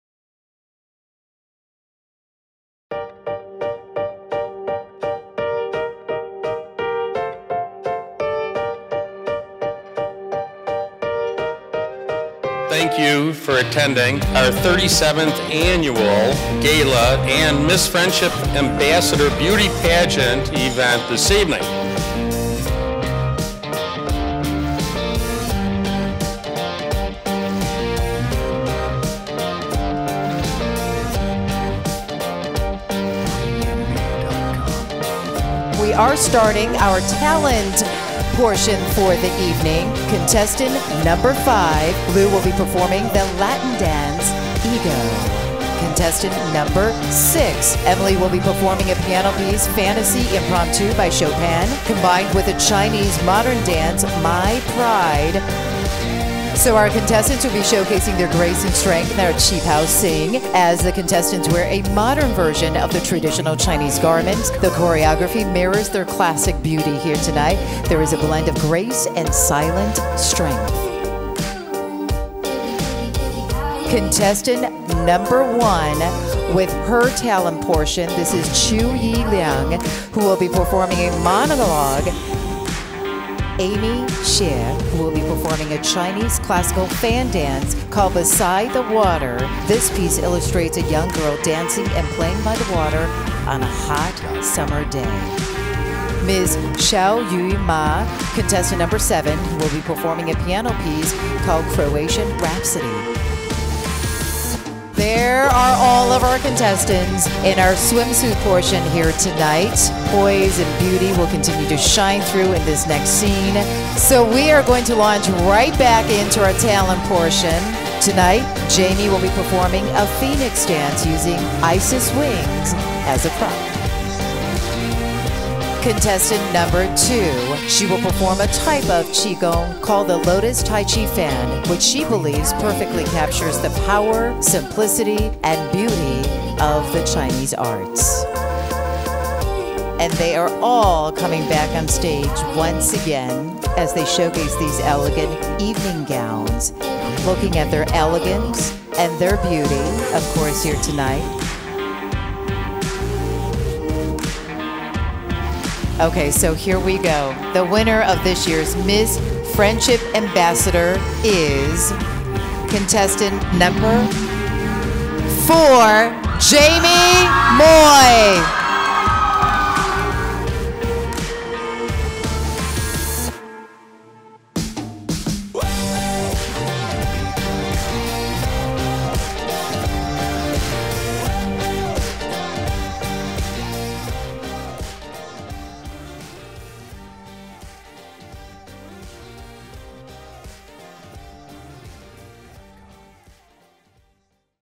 2019 Annual Gala & Miss Friendship Ambassador Pageant
The Annual Gala is the Chicago Chinatown Chamber of Commerce's major fundraising event. Celebrating and uniting the largest gathering of Chinese and American business leaders in the Chicago metropolitan area and the State of Illinois, including dignitaries, government officials and influential business and community leaders. As a designated City delegate agency, the Chicago Chinatown Chamber of Commerce strives to continue enhancing and expanding the Chicago Chinatown's businesses and networks.
The Miss Friendship Ambassador Beauty Pageant event is held every other year. This special event recognizes the beauty and talent of Chinese American women around the world, in addition to representing the future of the Chinese American community in Chicago, highlighting the merit of Chinese heritage, culture, and diversity.